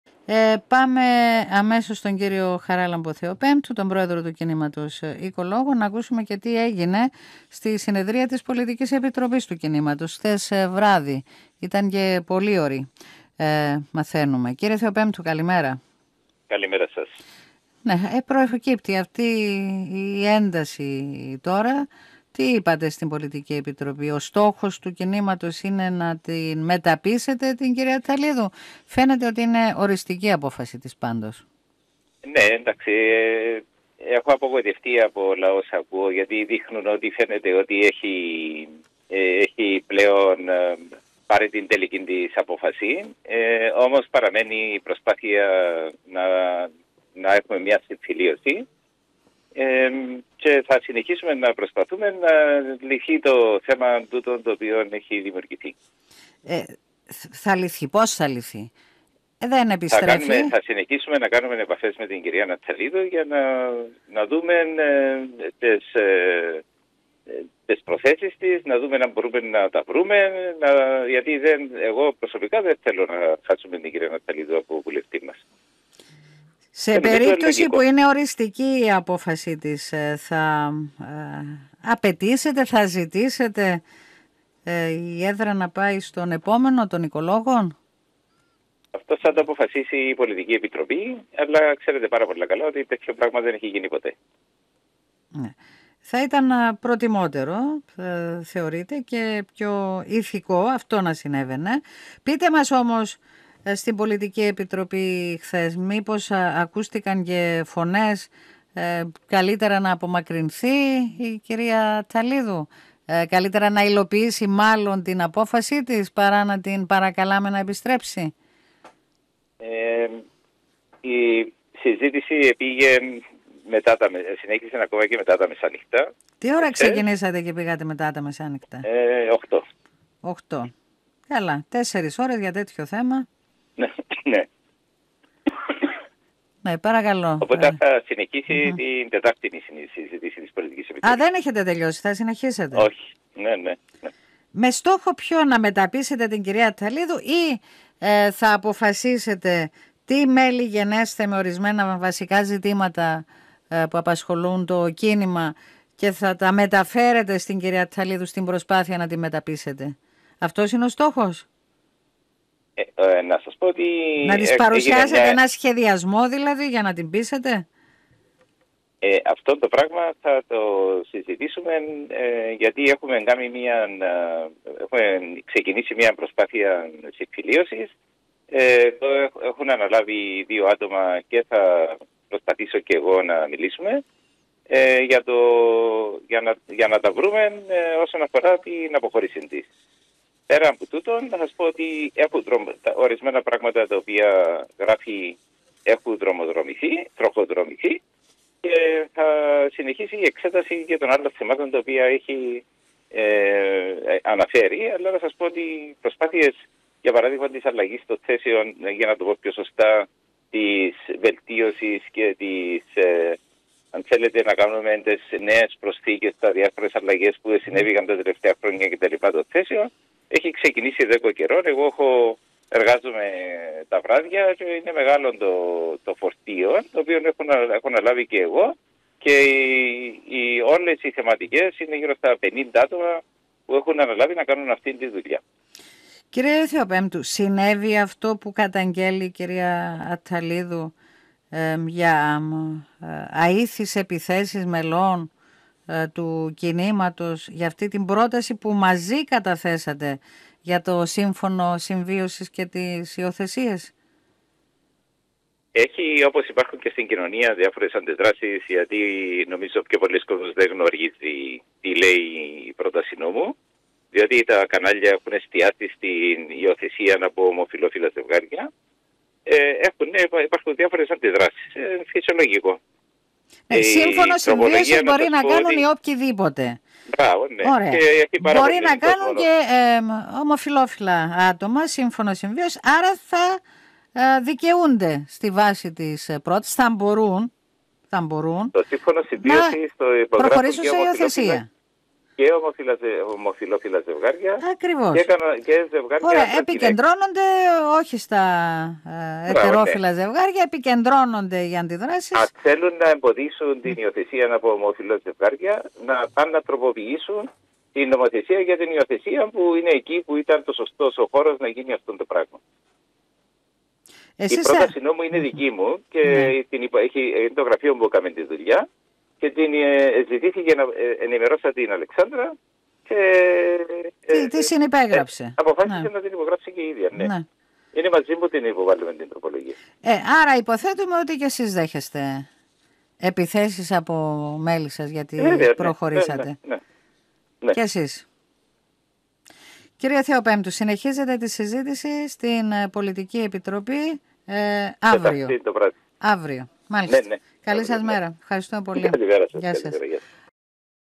17/10/23 – Ο Χαράλαμπος Θεοπέμπτου στο Τρίτο του ΡΙΚ για αποχώρηση της Αλ. Ατταλίδου
Ο Πρόεδρος του Κινήματος, φιλοξενούμενος στο Τρίτο του ΡΙΚ, συζητεί το θέμα της αποχώρησης της κ. Ατταλίδου, τις προσπάθειες που καταβάλλονται για συμφιλίωση καθώς και το θέμα της πρότασης νόμου για δικαίωμα υιοθεσίας παιδιών από ζευγάρια που έχουν συνάψει σύμφωνο συμβίωσης.